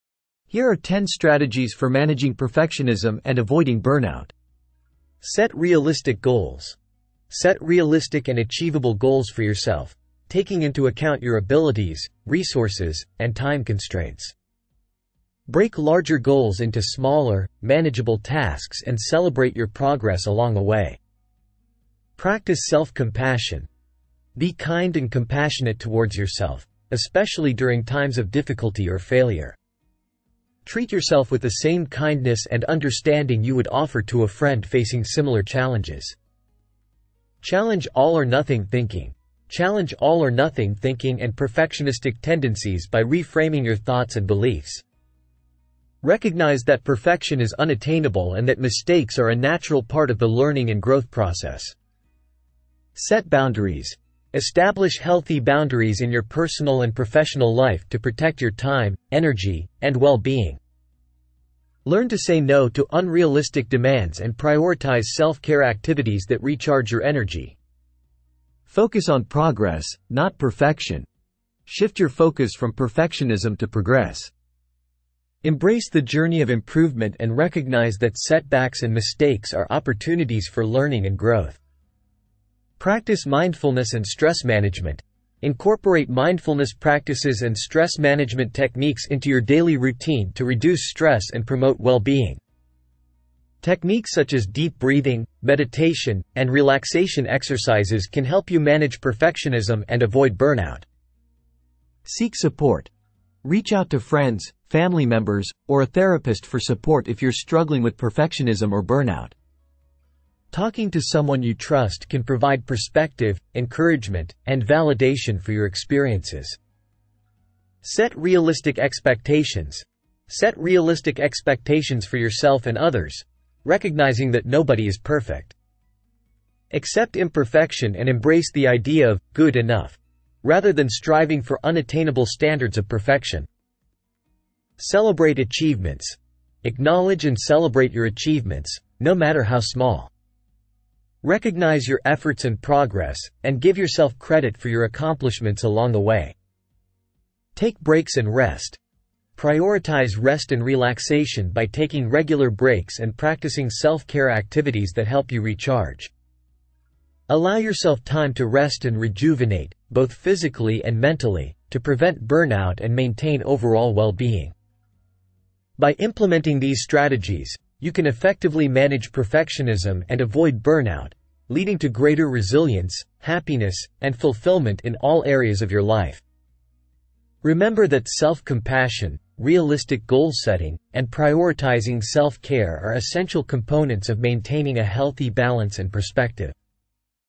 Top 10 Strategies for Managing Perfectionism and Avoiding Burnout
Manage perfectionism and prevent burnout with strategies like setting realistic goals, practicing self-compassion, embracing imperfection, seeking support, and prioritizing self-care to maintain well-being and productivity.